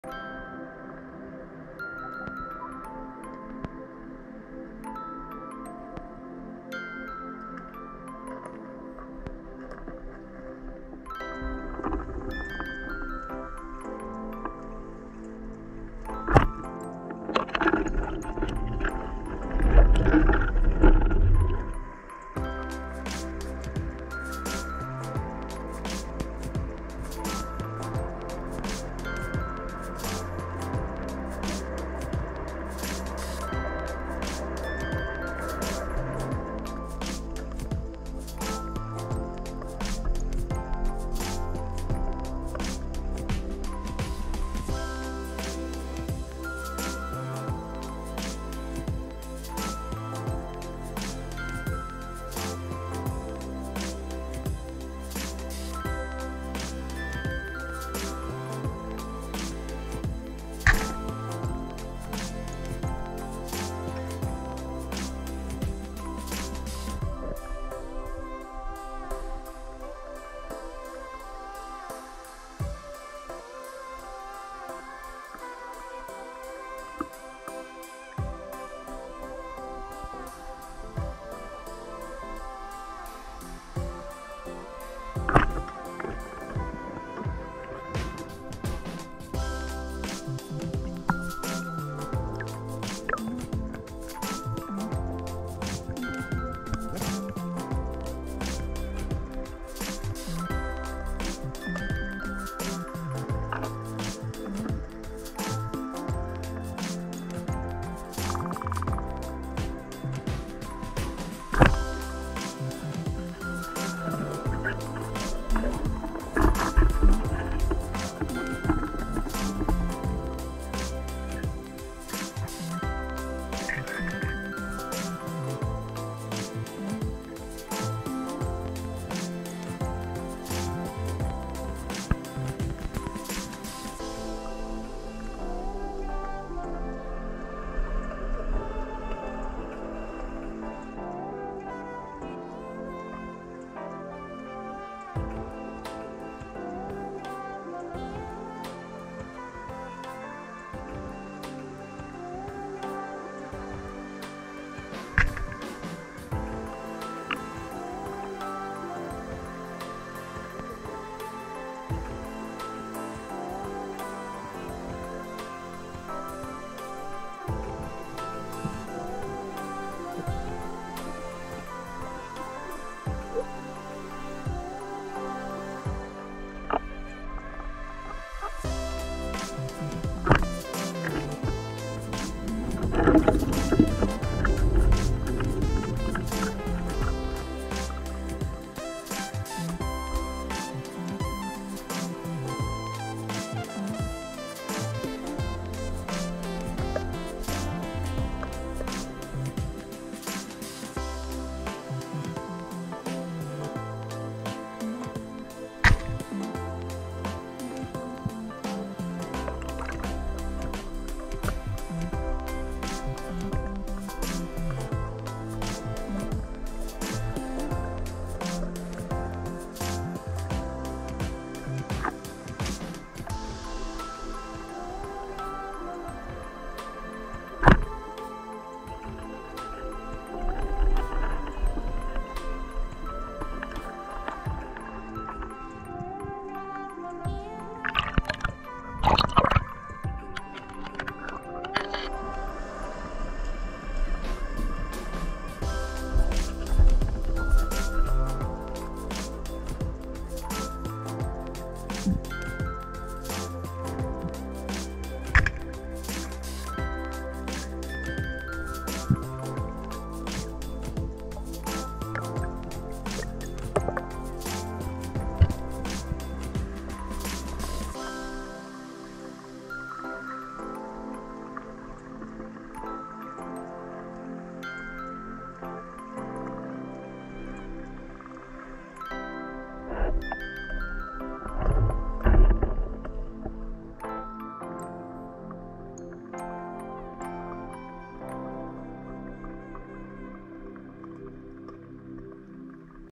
Spearfishing Norway, October 2021
Compilation of six spearfishing trips this October. Plenty of cod and pollack, and more rare encounters such as lumpfish and a cormorant swimming by. All clips are from the area around Molde, Møre og Romsdal.
All recordings were made with Gopro Hero 7 Black.